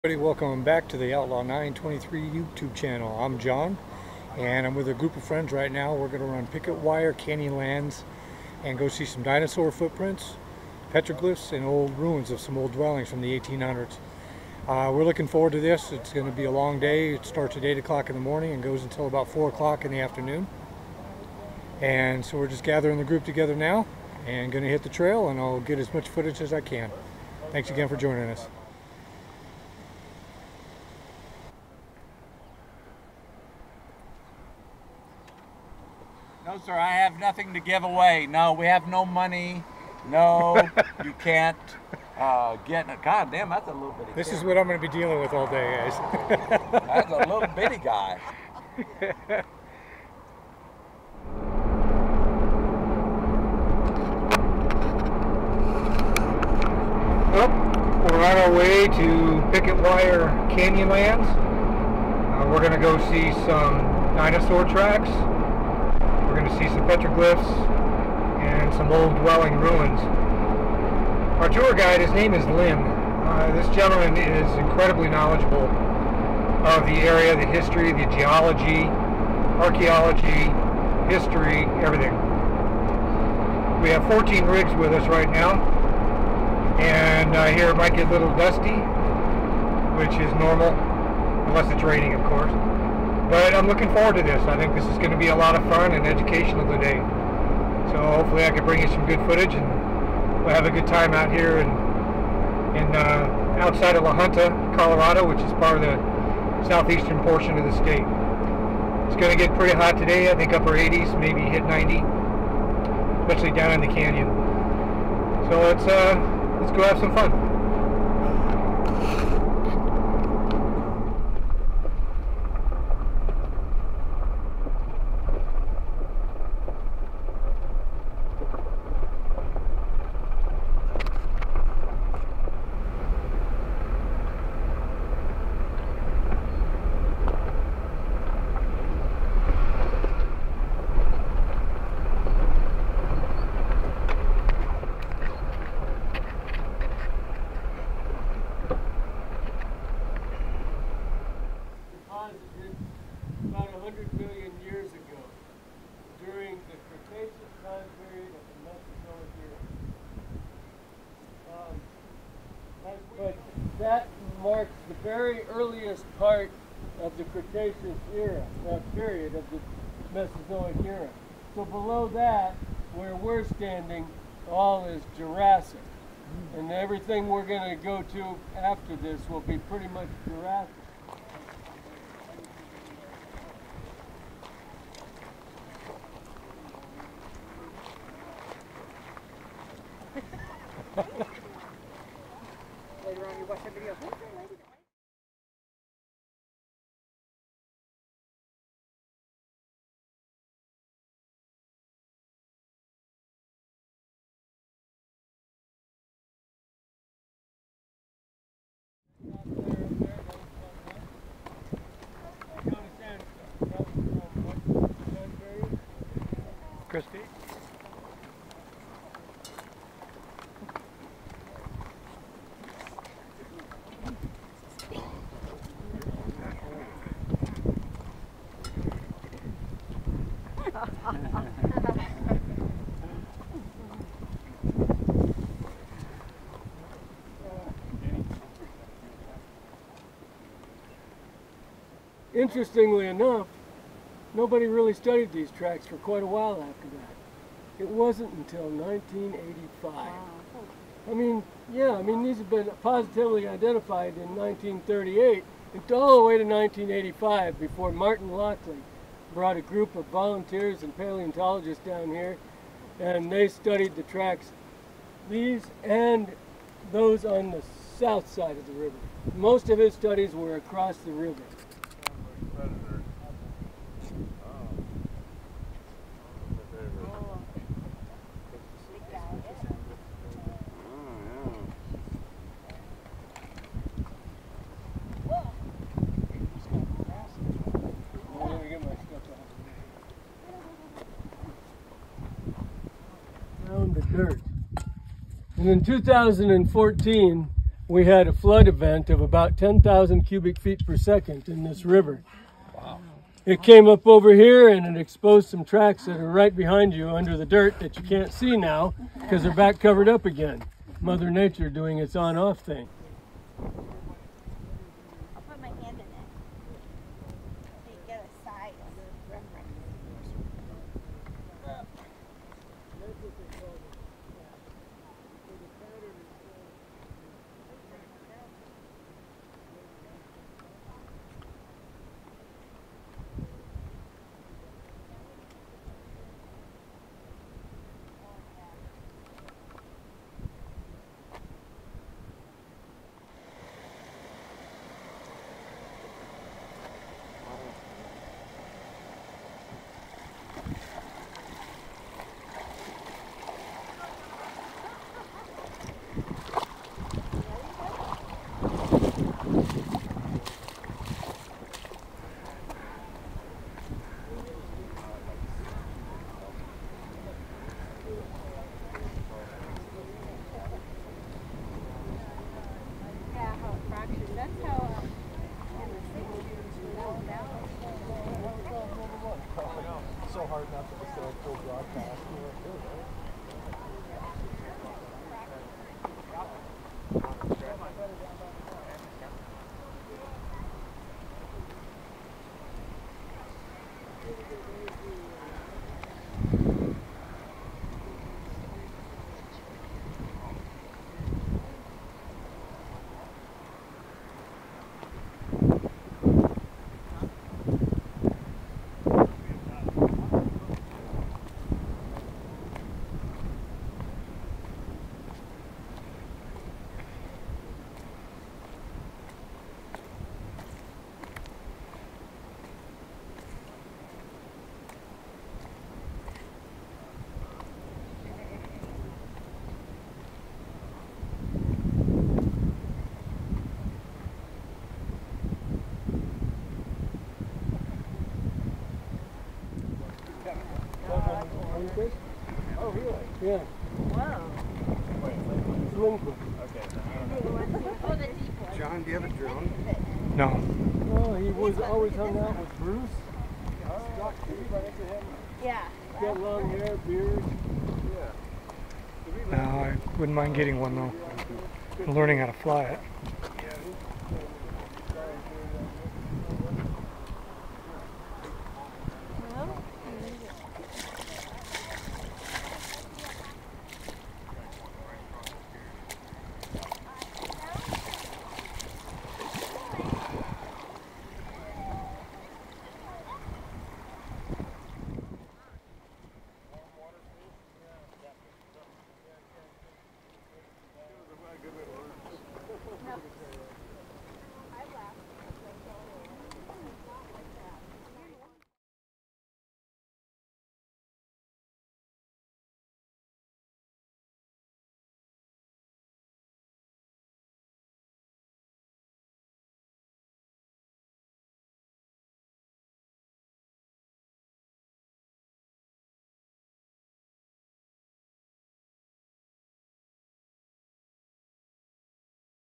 Dinosaur Tracks Picket Wire Canyonlands.
This dinosaur track site is the largest found in North America.  There are over 1500 prints and 100 separate trackways. The prints are over 150 million years old.  Auto tours are conducted by the USDA Forest Service in the Comanche National Grassland in La Junta, Colorado. The track site itself is south of La Junta outside of Kim, Colorado.  It's worth checking out and Lyn was an incredible tour guide!